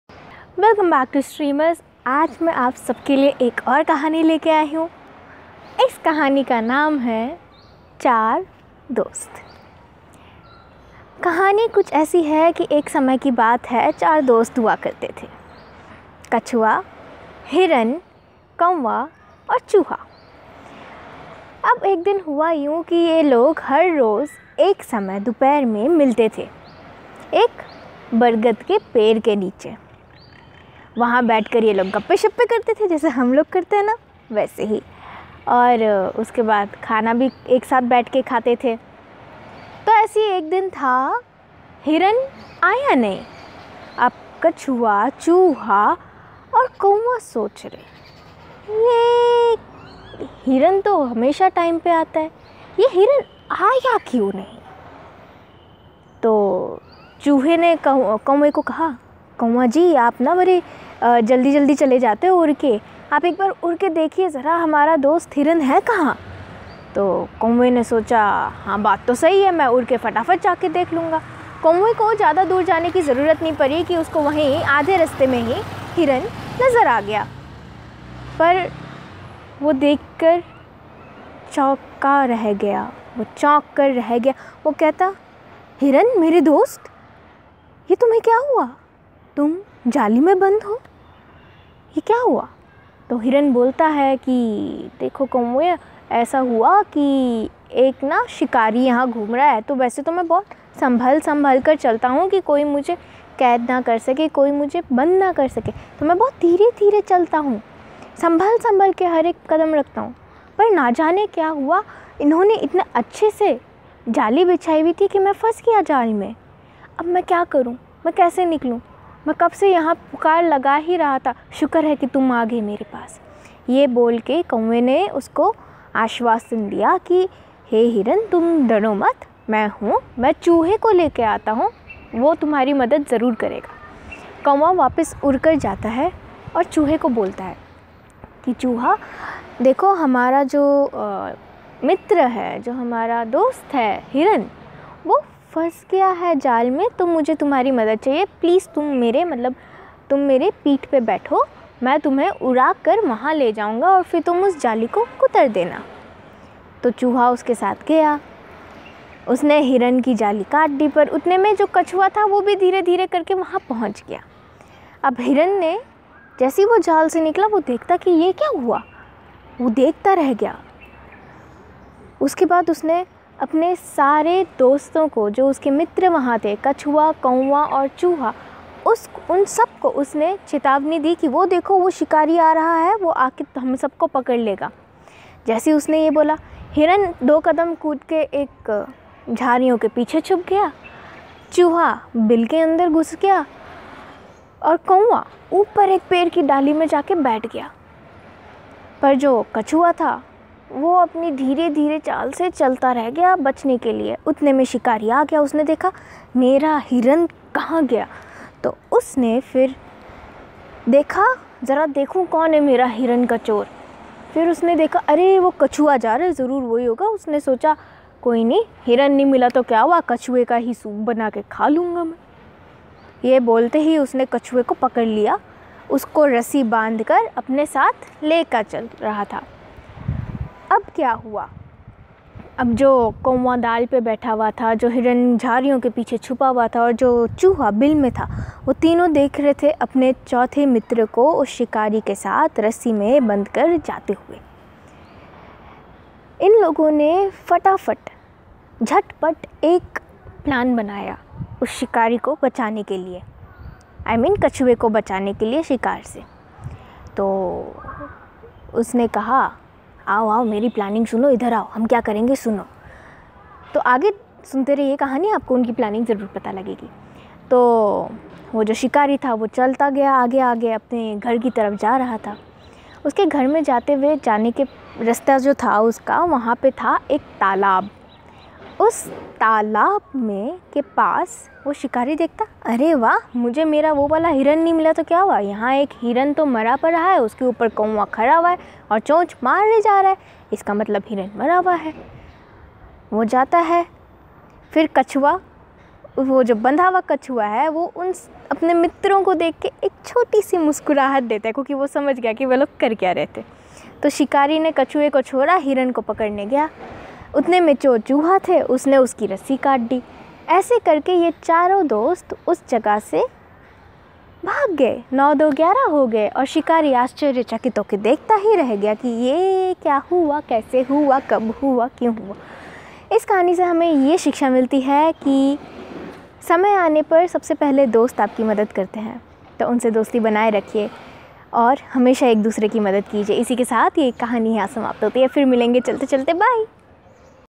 Chaar Dost (Hindi)| Moral Story #7| Jeevan Darpan#57
These stories will 1st come in Hindi and then in English. So keep Watching!!
The story has been taken from Panchatantra.